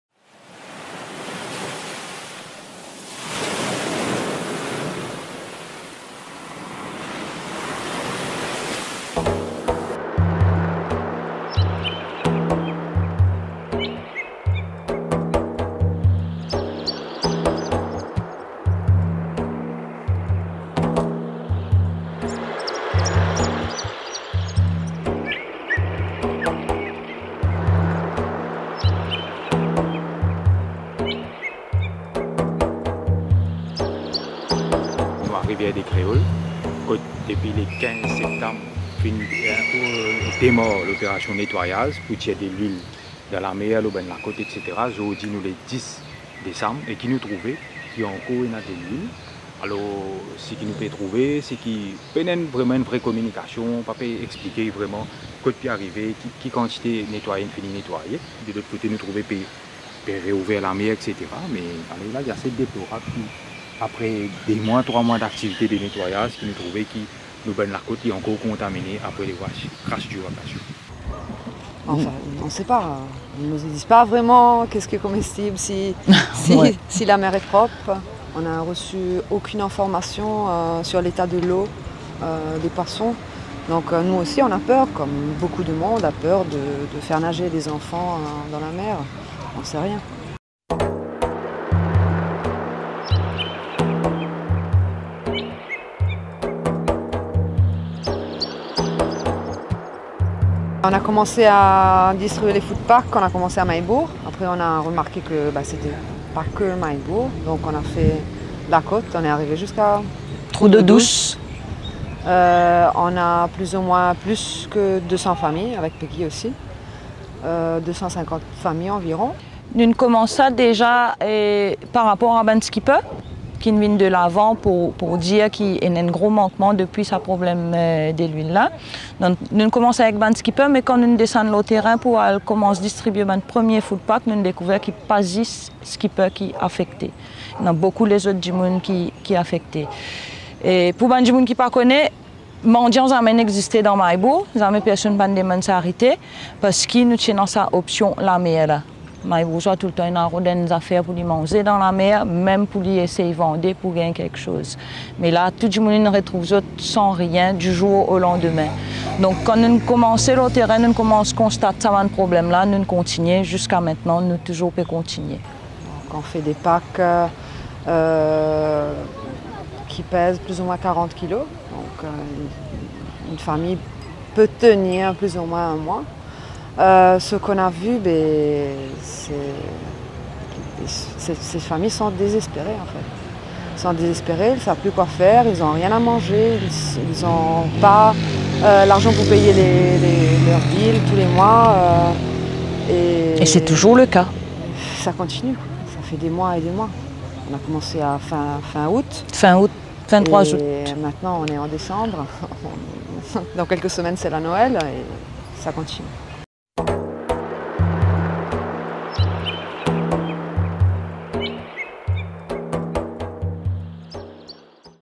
Surviving Wakashio...
The Wakashio vessel that wrecked in Mahebourg lagoon has created a lot of despair. Virginie Orange, Laura Morosoli and Jean Michel Rose have been busy since the very beginning of this crisis in giving much of their time and energy to support some 200 families whose livelihoods depend in a way or another on the sea. Even if the government of Mauritius is, for some reason, playing it cool as if everything is back to normal, such is not the case. Families are still in distress and traces of oil are still visible. So far no official data and figures that indicate the toxins that are still in the water... yet the sea has been opened again after several months for leisure activities...